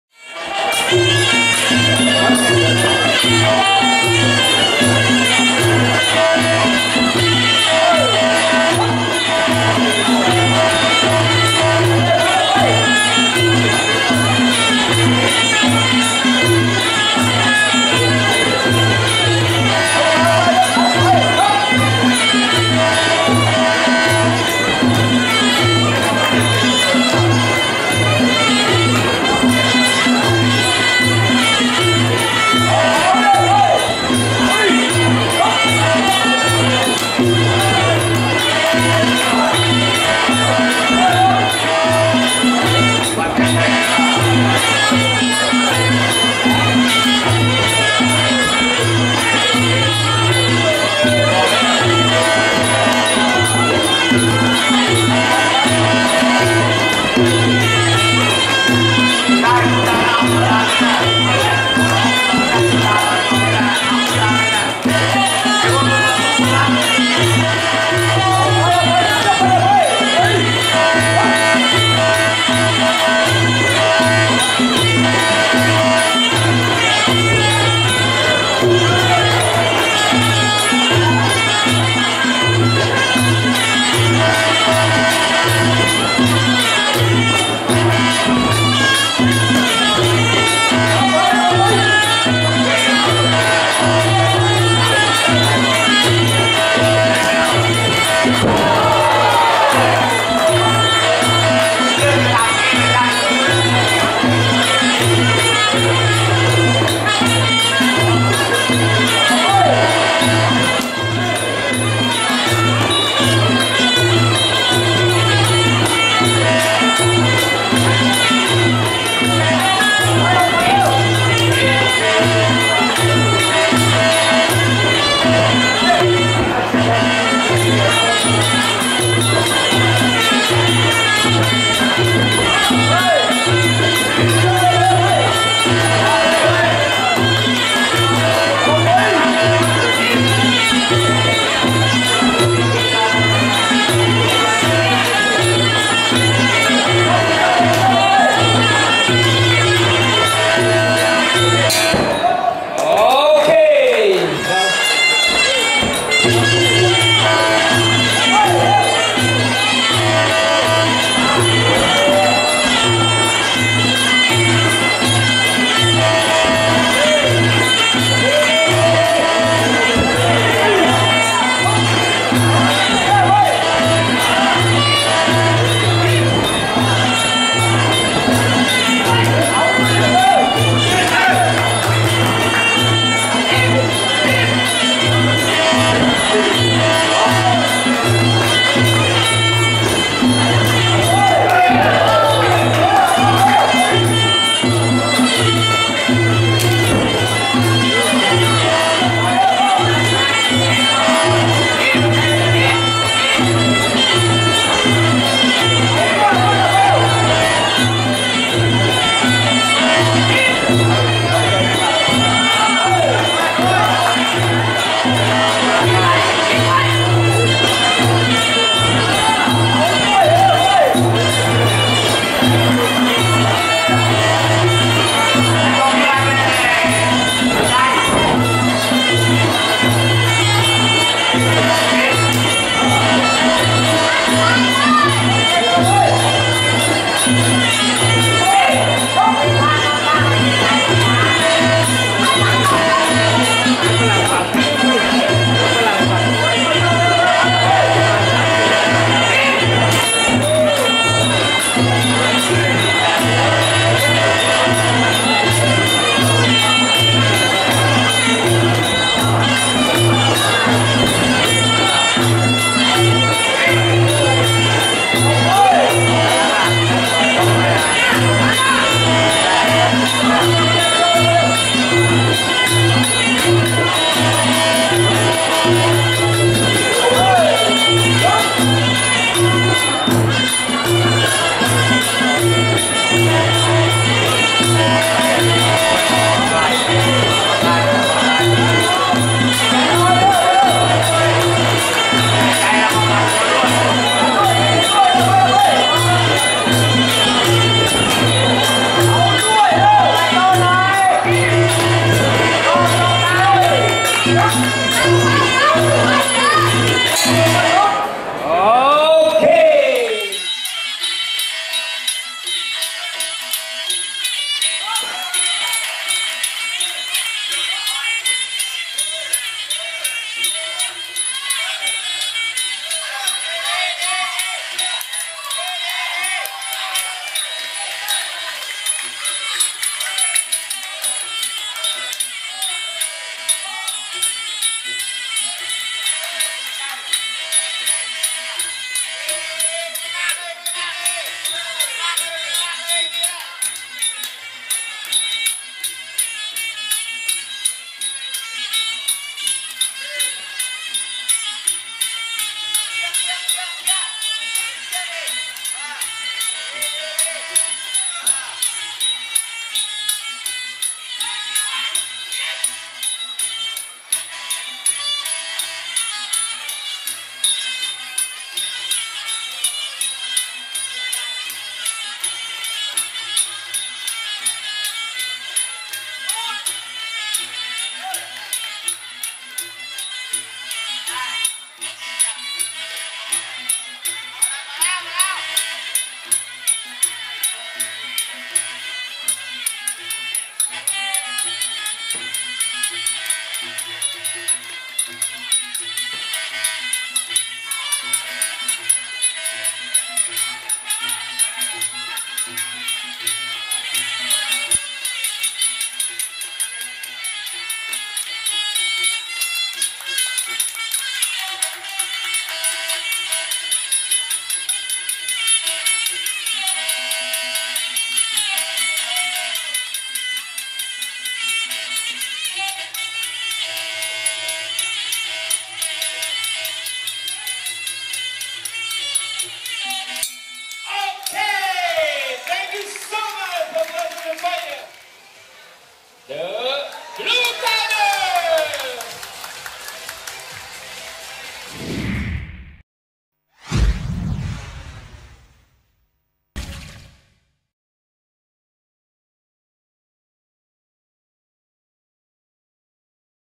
Lukchai vs Ratkeeree @ BBQ Beatdown 100
We celebrated the 100th edition of our monthly BBQ Beatdown party this past Saturday. This event featured an all you can eat buffet, drinks, DJ and amateur Muay Thai and MMA fights as usual, as well as pro Muay Thai fights.
In this pro Muay Thai fight, Lukchai and Ratkeeree face off.